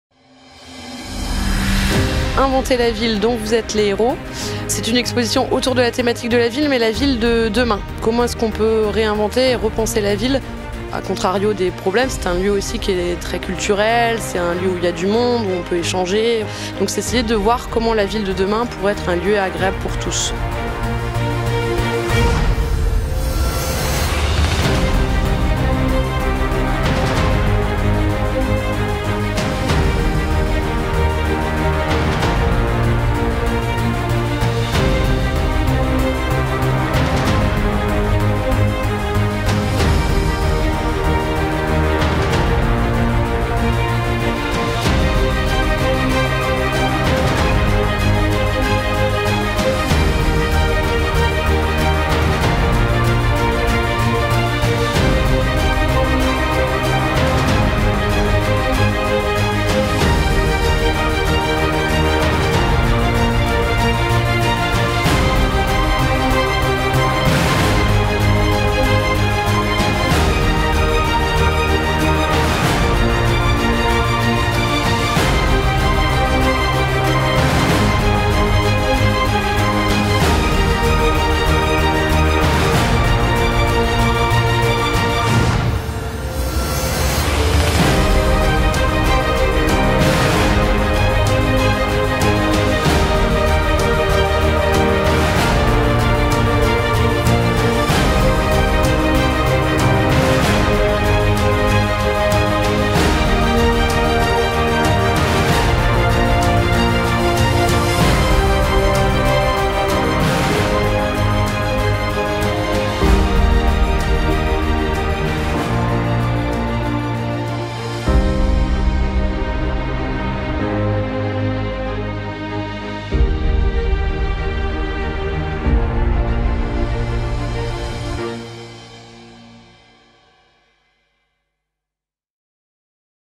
Inventez la ville, dont vous êtes les héros !
Découvrez l'exposition "Inventez la ville, dont vous êtes les héros !" co-produite par Science Animation et Toulouse métropole.

Qu’entend-on par « ville » ? Comment évolue-t-elle ? Quels sont les différences et les points communs à travers le monde ? Quels sont les métiers qui « fabriquent » la ville ? Quelles sont les innovations de demain ? De quels outils sont dotés les citoyens pour influencer leur ville ?...
Inventez la ville… dont vous êtes le héros ! propose de découvrir tout cela et plus encore, à travers une immersion au cœur des villes du monde et leurs multiples facettes. Adaptée à tout âge, l’exposition balaye le passé, le présent et le futur de la ville, en offrant au visiteur un véritable voyage interactif.